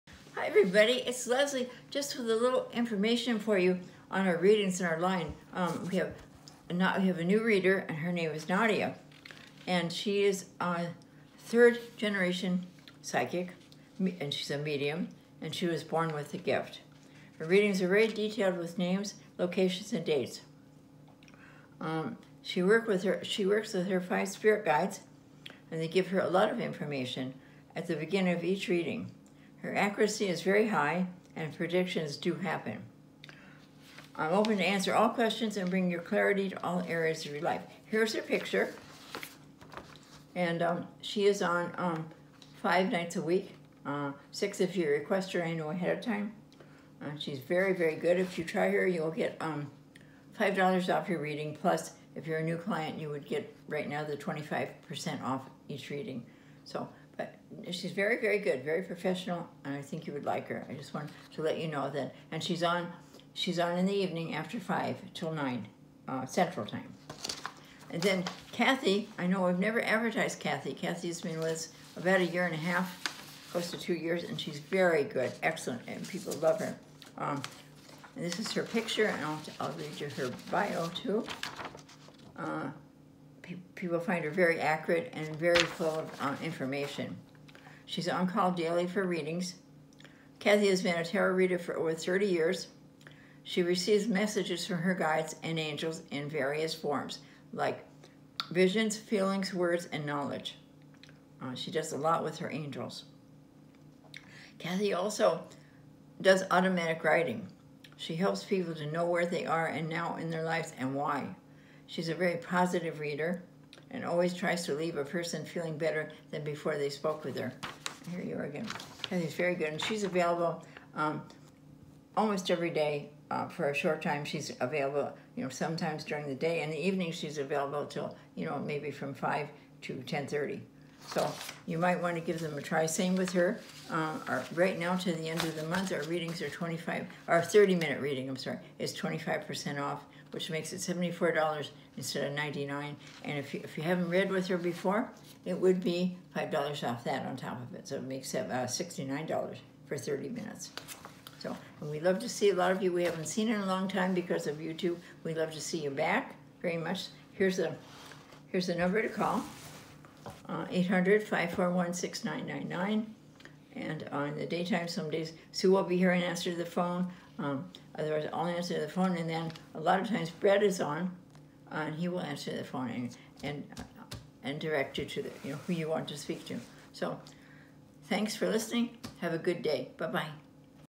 A look at Our New Readers!
Please help us welcome our new readers- Nadia and Kathy!

"My name is Nadia. I am a third Generation Psychic/ Medium. I was born with the Gift.
My readings are very detailed with Names , Location , Dates etc ...I am a No tool reader I work with my 5 Spirit Guides who give me lots of Information at the beginning of Each Reading. My Accuracy is very high & My predictions DO happen! I'm open to Answer All questions and Bring you Clarity in All areas of your Life."
"I have been a tarot reader for over 30 years. A lot of my reading is intuitive and I receive messages from my guides and angels in various forms like visions, feelings, words and knowledge. I also do automatic writing. I help people to know where they are now in their lives, and why. I am a very positive reader and I always try to leave a person feeling better than before they spoke with me."
- Kathy

Want to schedule a reading with Nadia or Kathy? Visit psychicleslee.com for more information or to schedule a reading.